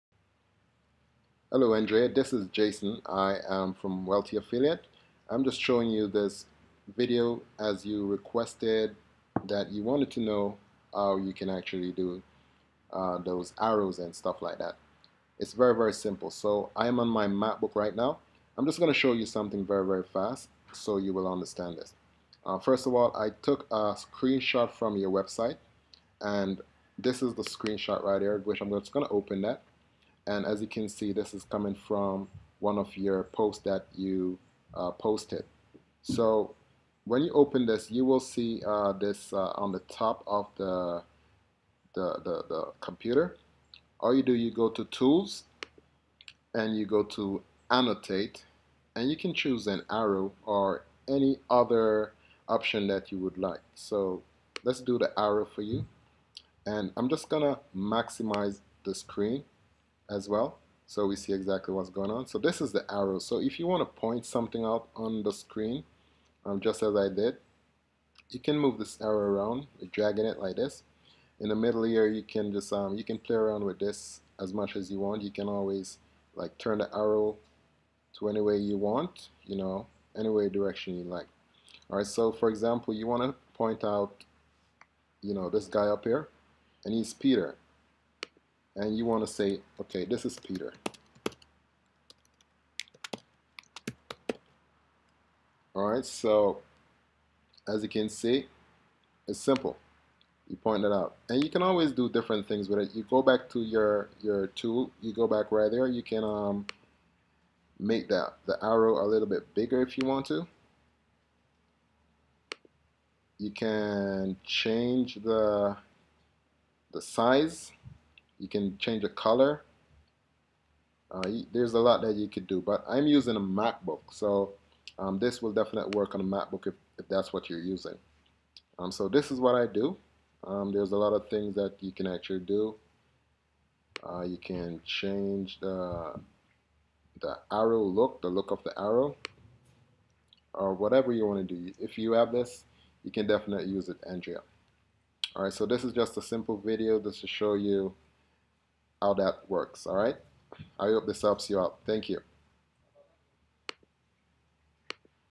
Arrow Image - Learn How Easily
I was asked by Andrea, How To Insert An Arrow To An Image, So I made This Video To Show Him.

You may also want to know how to do this.

I use a Macbook Air, so this will not work on a PC.

I hope That you found some value from this.

Jason.